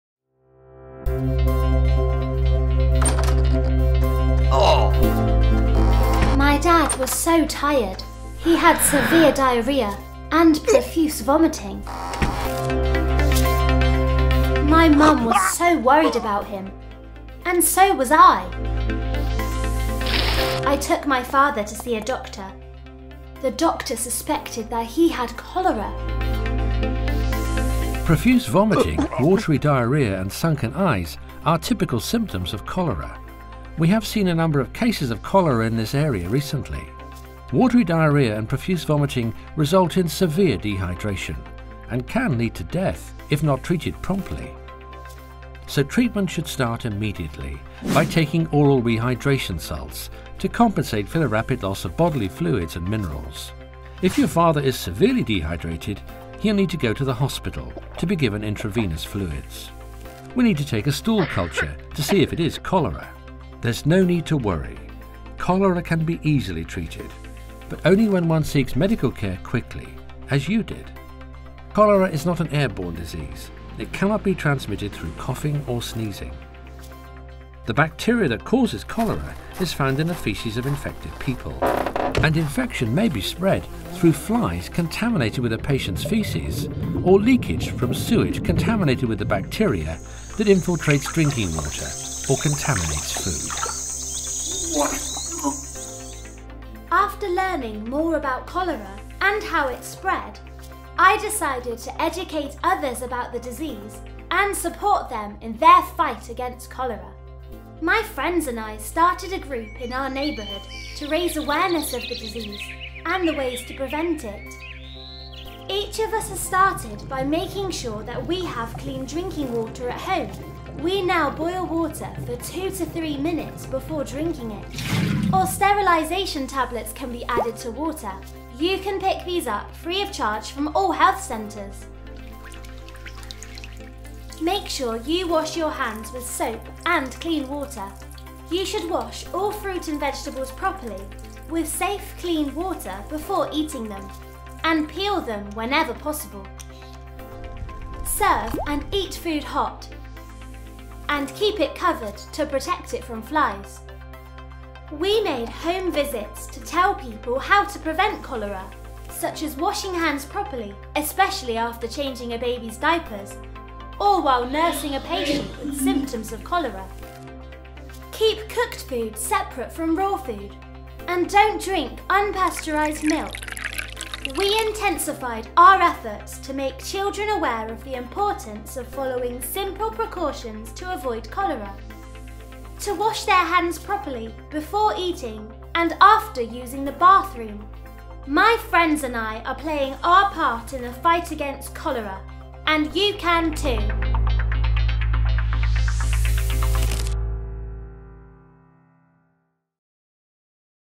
Cholera – you can fight it too!
Cholera is an acute diarrhoeal infection contracted by consuming food or water contaminated with the bacterium Vibrio cholerae.  It affects both children and adults and can kill within hours if not treated.

A short animated video that provide's information about Cholera, symptoms, transmission, signs, treatment and what could be done to prevent contamination in a simple and dramatic style.

Concept & production by YAT Communication for WHO-EMRO